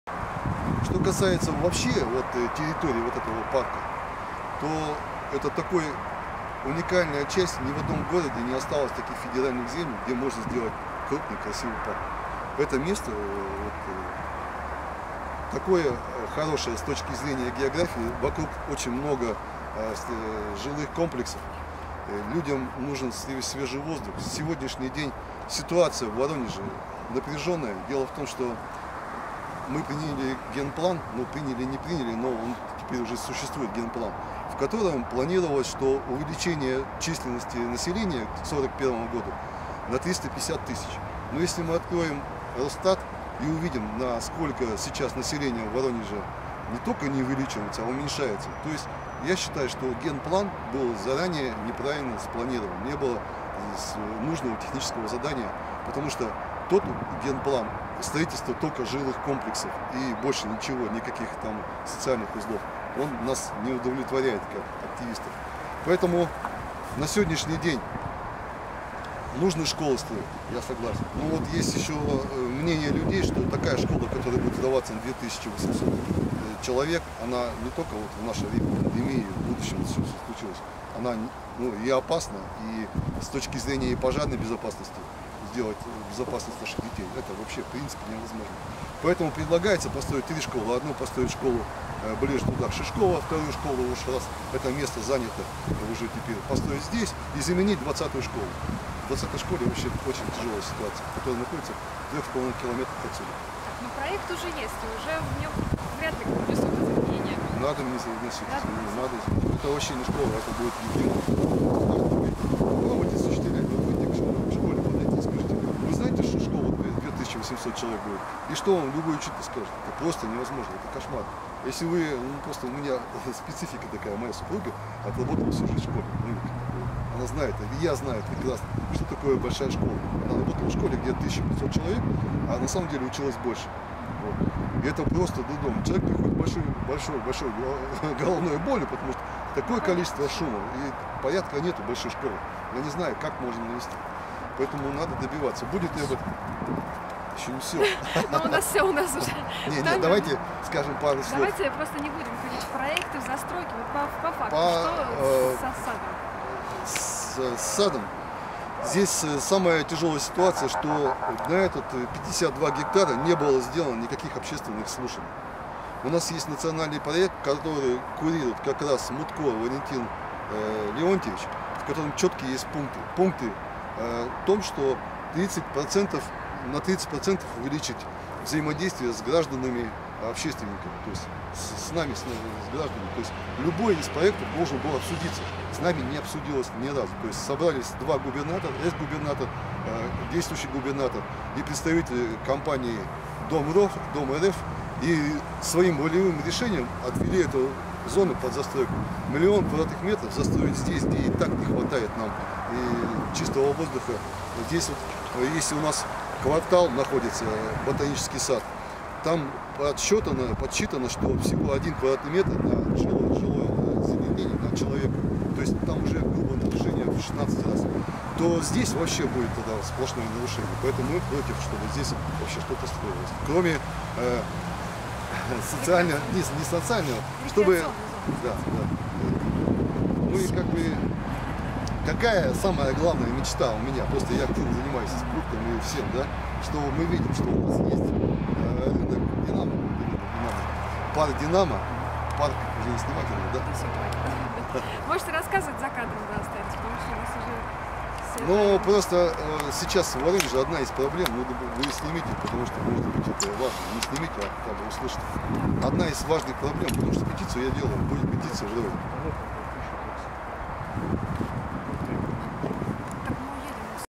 Создание парка на территории Яблоневого сада (Воронеж) вместо плотной застройки
На территории Воронежского яблоневого сада, где кроме яблоневых деревьев большое количество сосен, берез, дубов и кленов, не было никаких общественных обсуждений по застройке 52га. Решение было принято компанией Дом.рф без каких-либо обсуждений... Люди уже долгое время хотят видеть парк на этой территории и полномасштабный опрос показывает, что 93% (из более 1000 проголосовавших) за создание парковой территории вместо плотной застройки. Официальная петиция на сайте Российской Общественной Инициативы как раз за это –